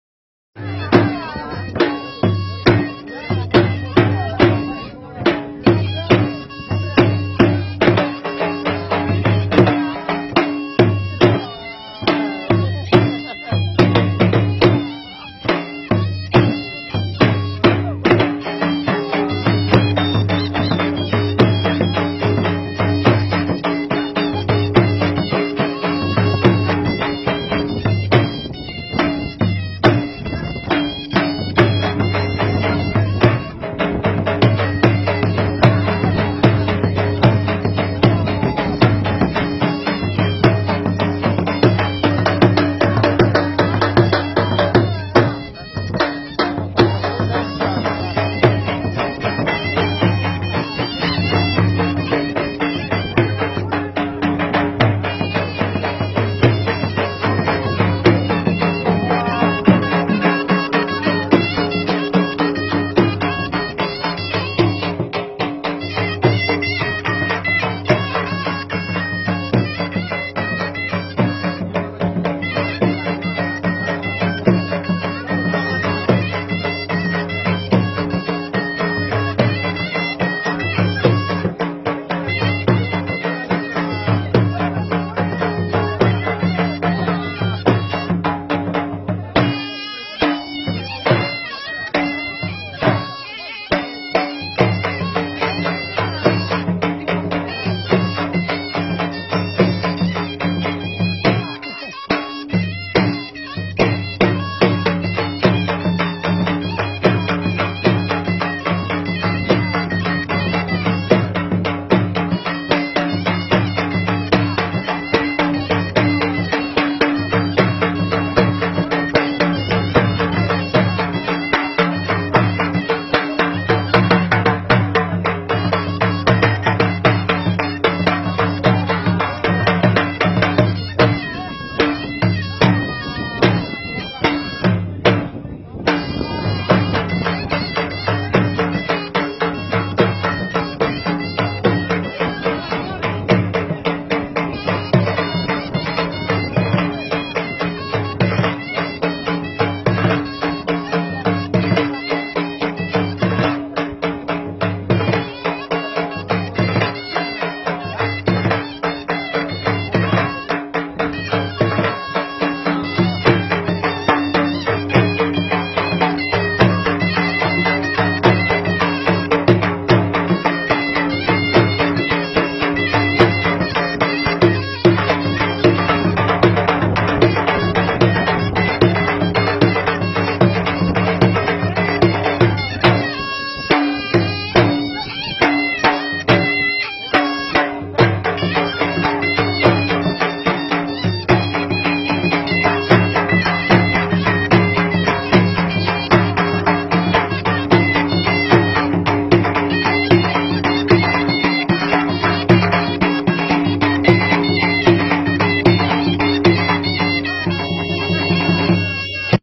Small. boy,s dancing, in wedding.party
Pakistan,s wedding dance culture and party;s- thanks to manoj kumar birav --babu moalla mastung